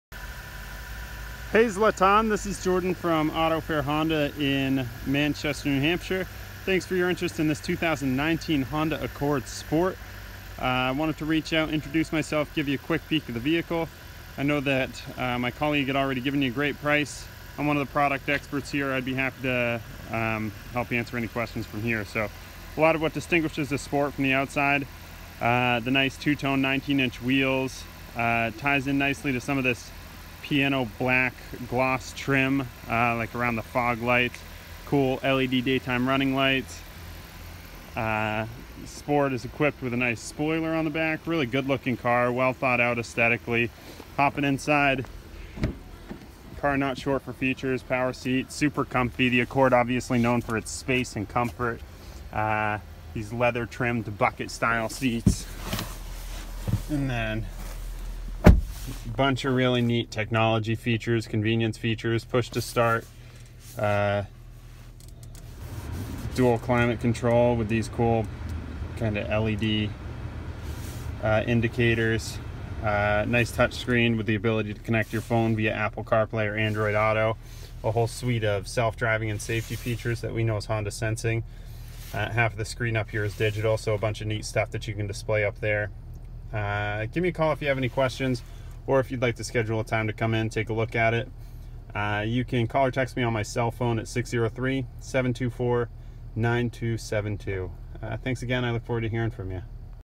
Zlatan, thanks for your interest in an Accord Sport
Hi, Zlatan. This is Jordan from AutoFair Honda in Manchester. Thanks for your interest in a new 2019 Accord Sport. Here’s a brief video I recorded just for you. Please let me know if you have any questions; I’m happy to help.